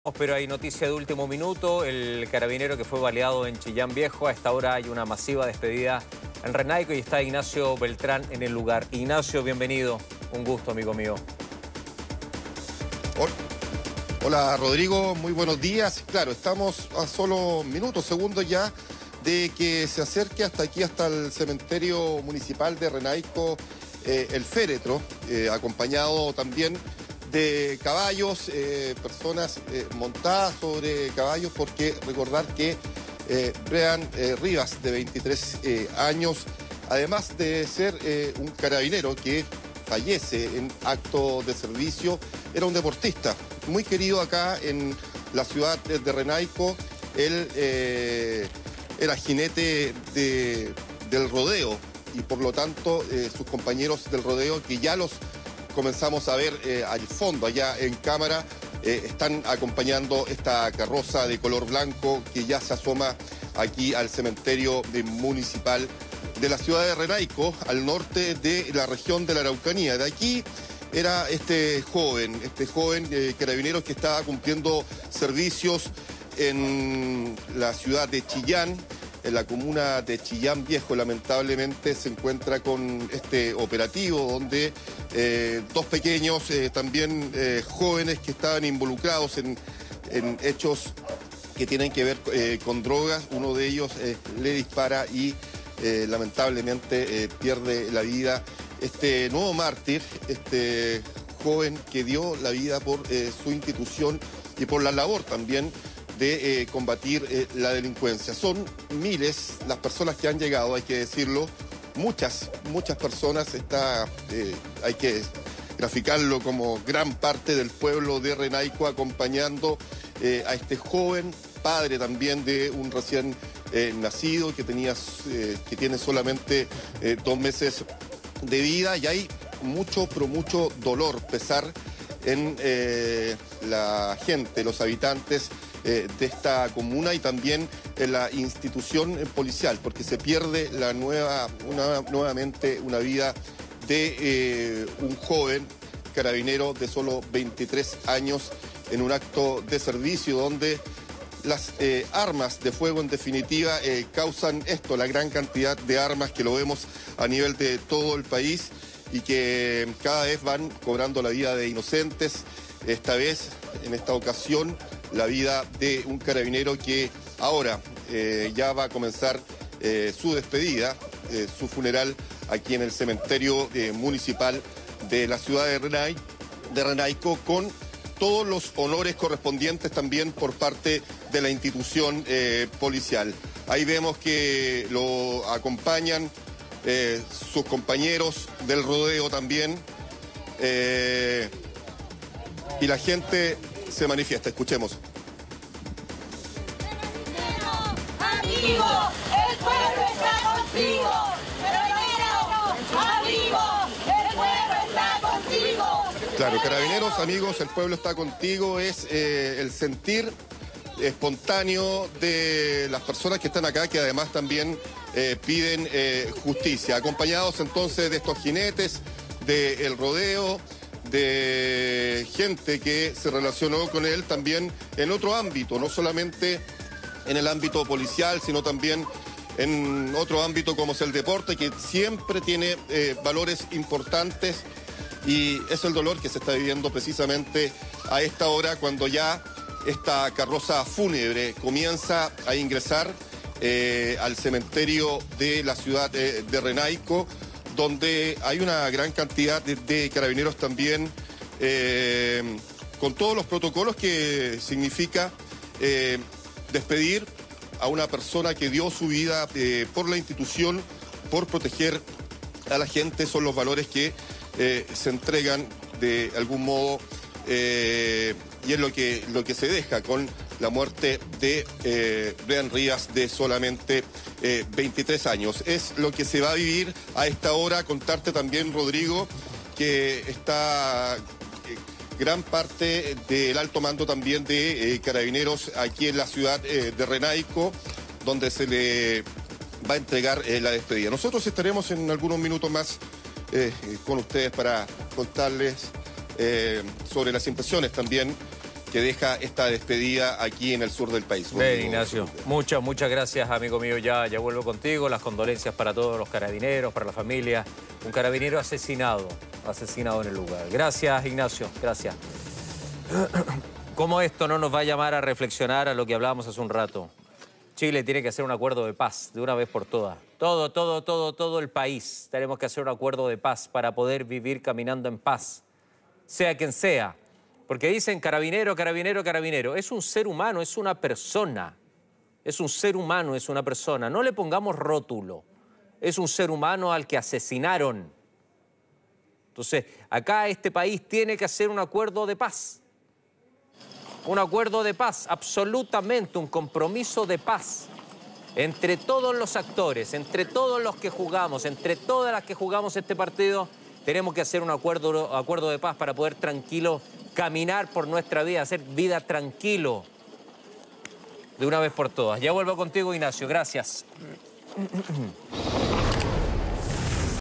Funeral del carabinero Breant Rivas: Renaico despide y homenajea al mártir de la institución
La comuna natal del joven carabinero salió a darle el último adiós en una despedida cargada de homenajes.